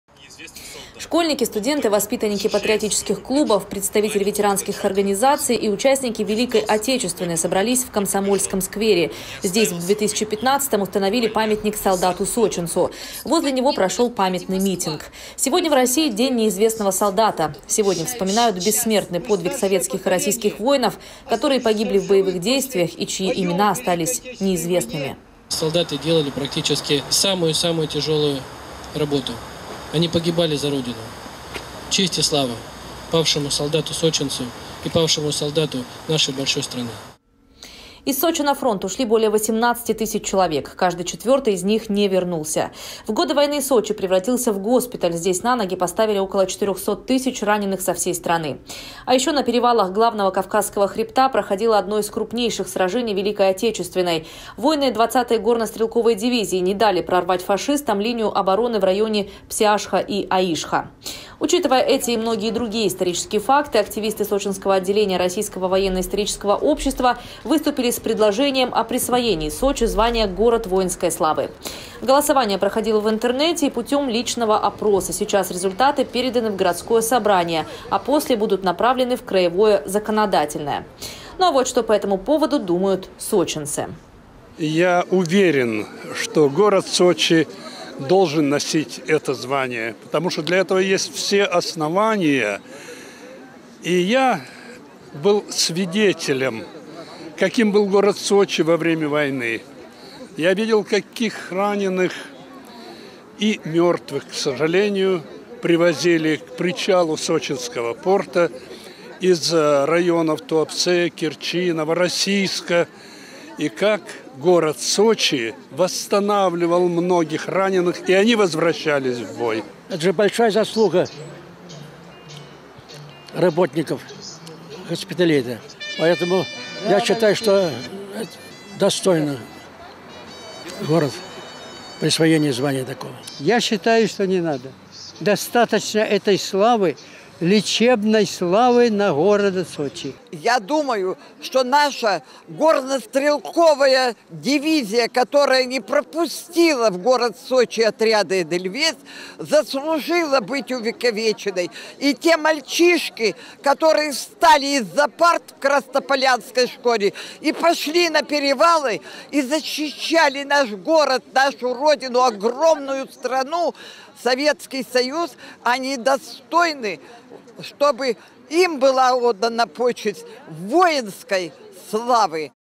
В Сочи состоялся митинг памяти в честь Дня неизвестного солдата. Новости Эфкате
Школьники, студенты, воспитанники патриотических клубов, представители ветеранских организаций и участники Великой Отечественной собрались в Комсомольском сквере.

Здесь в 2015-ом установили памятник солдату-сочинцу. Возле него прошел памятный митинг. Сегодня в России – День неизвестного солдата. Сегодня вспоминают бессмертный подвиг советских и российских воинов, которые погибли в боевых действиях и чьи имена остались неизвестными.